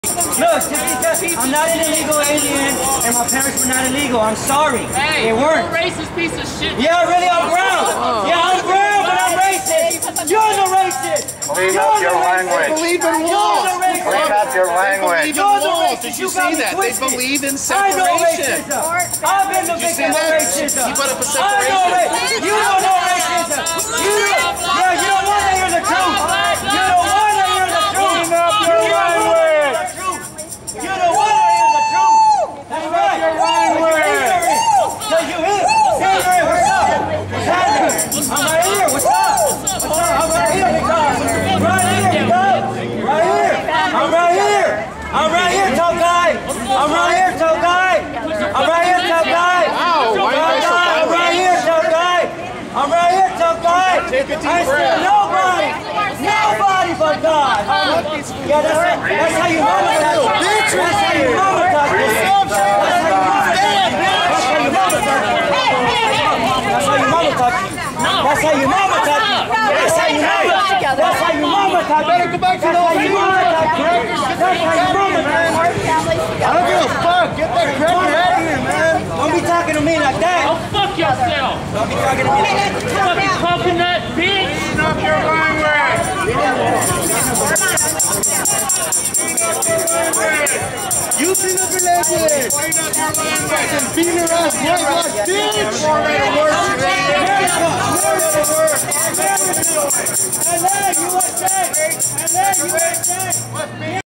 YIKES! Angry Pro Illegal Protesting ICE Mimes Pleasuring Himself Directed Toward ICE SUPPORTERS
7/23/18 Los Angles, CA  ICE protesters in front of the LAPD Headquarters clashed with ICE supporters.  The LA County Sheriff McDonnell had scheduled a debate with his opponent at the LAPD auditorium. The public was invited. When the police association claimed that protesters were planning on attending the debate, they cancelled all public tickets and stated the event was private for law enforcement only and other invited attendees.